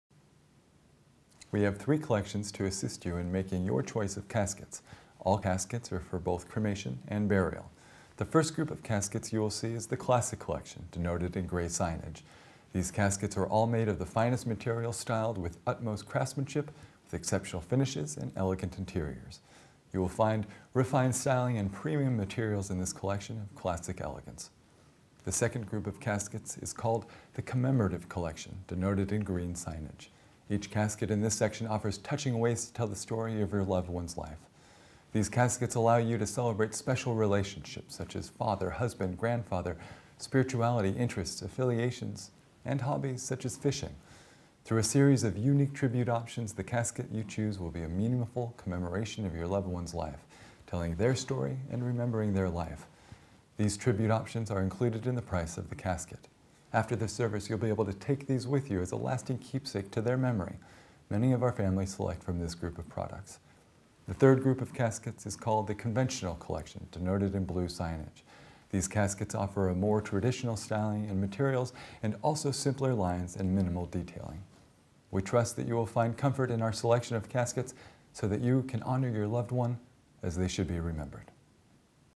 Casket Selections
At Mcinnis & Holloway we know that every life story is different and we want to help you tell your story. We have three collections to assist you in making your choice of caskets for both cremation and burial.